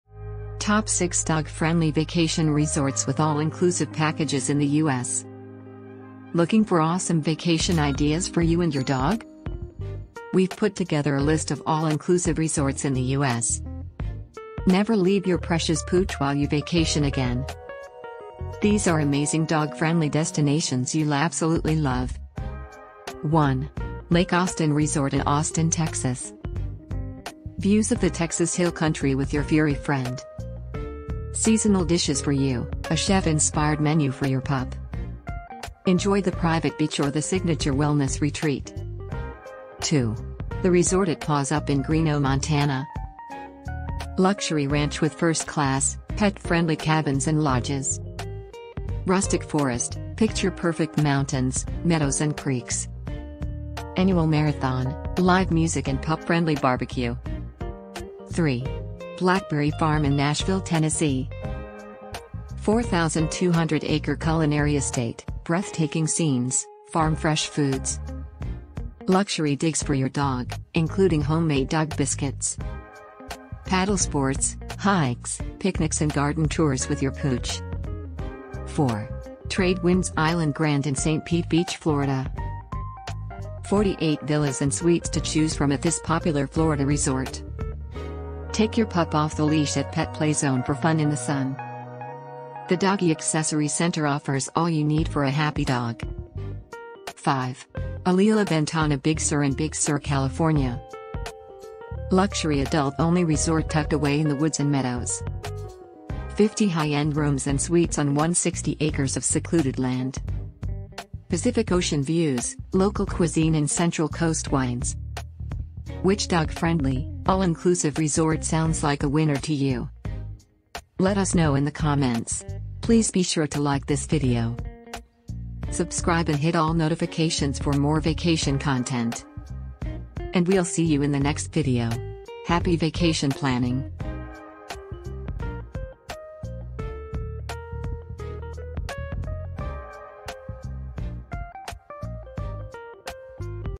Top 5 Dog Friendly Vacation Resorts with All Inclusive Packages
Looking for awesome vacation ideas for you and your dog?
We’ve put together a list of all-inclusive resorts in the U.S.
Never leave your precious pooch while you vacation again.
These are amazing dog-friendly destinations you’ll absolutely love.

1. Lake Austin Resort in Austin Texas
2. The Resort at Paws Up in Greenough Montana
3. Blackberry Farm in Nashville, Tennessee
4. TradeWinds Island Grand in Saint Pete Beach Florida
5. Alila Ventana Big Sur in Big Sur California